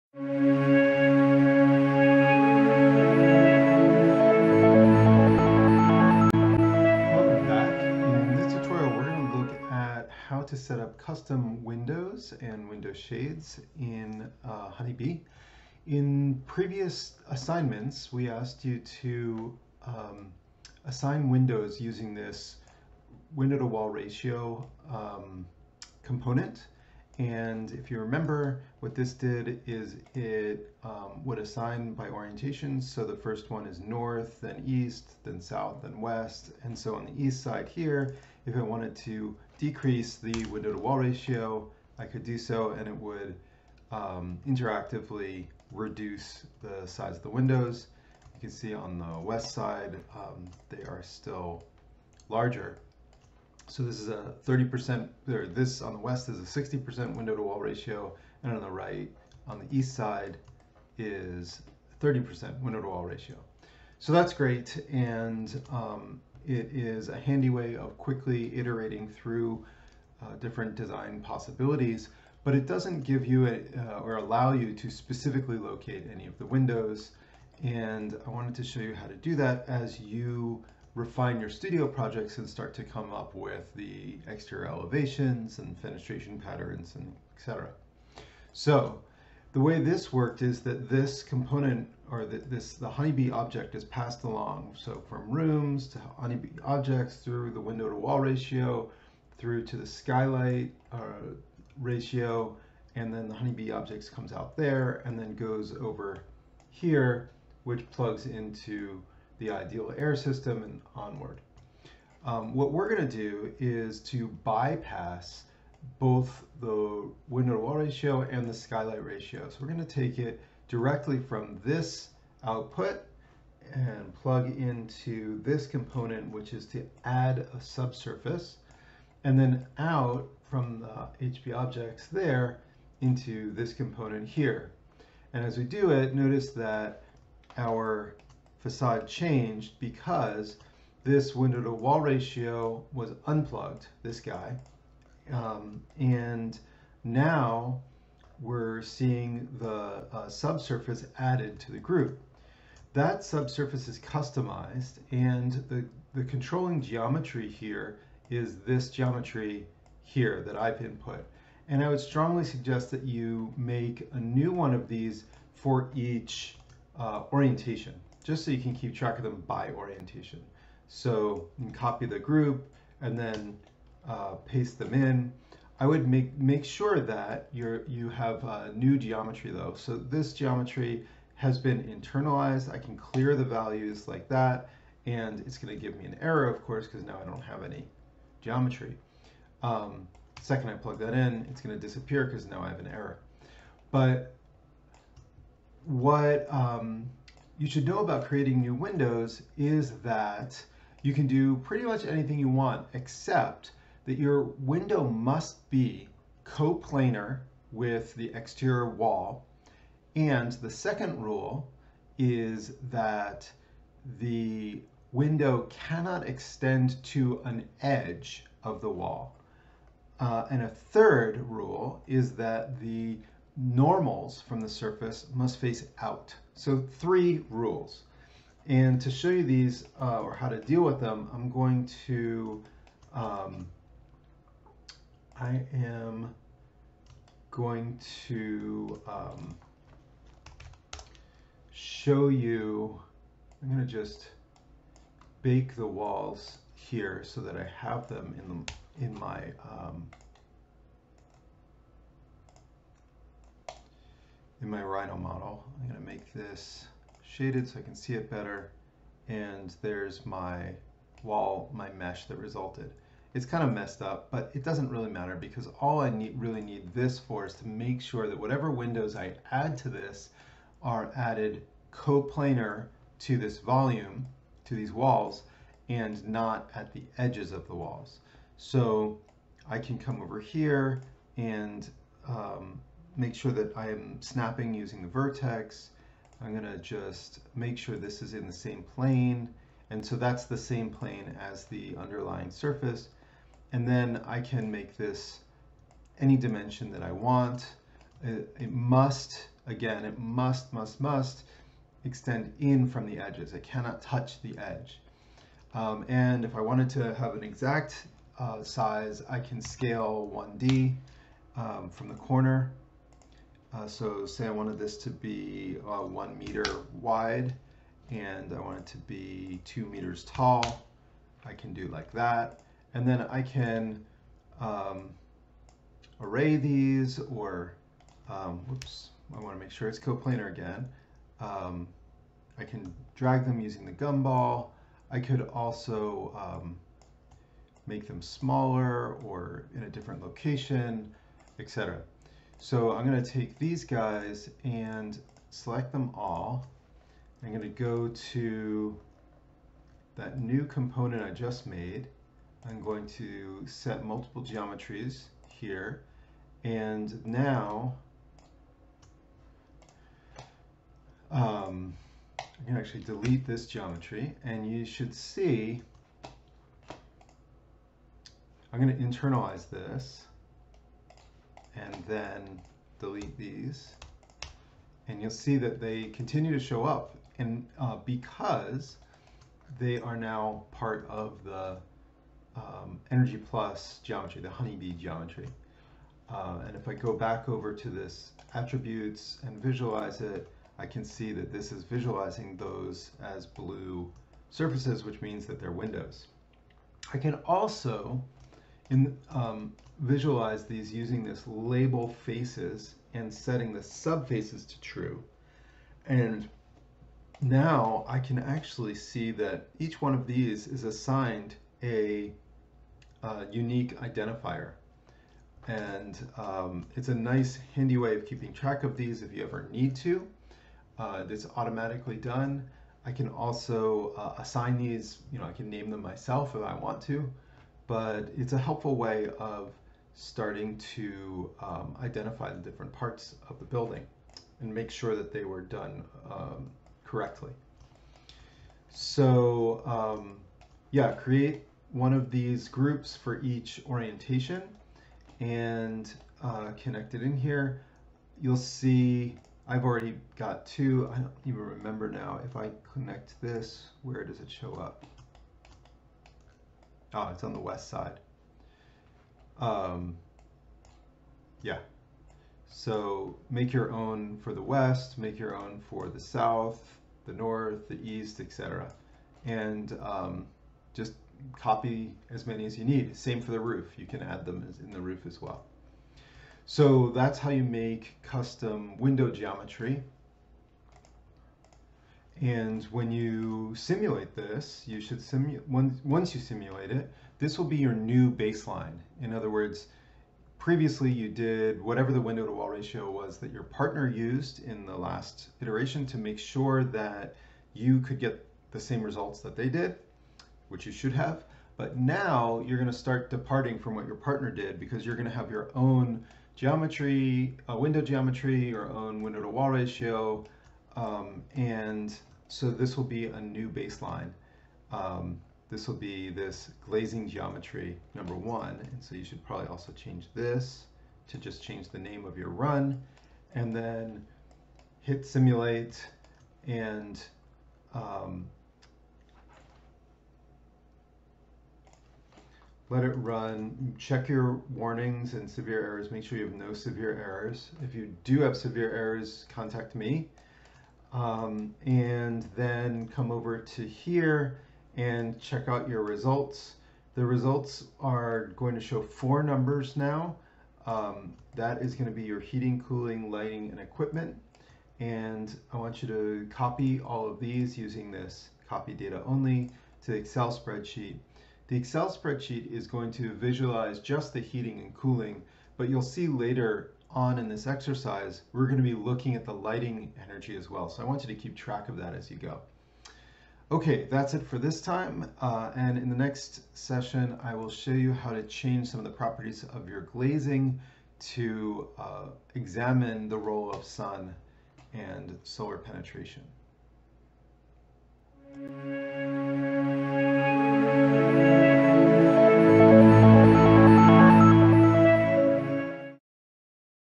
Honeybee V1.4 :: Custom Window Geometry [Tutorial 3.2]
This tutorial is intended for use with my class on Regenerative Building Performance at California College of the Arts in San Francisco.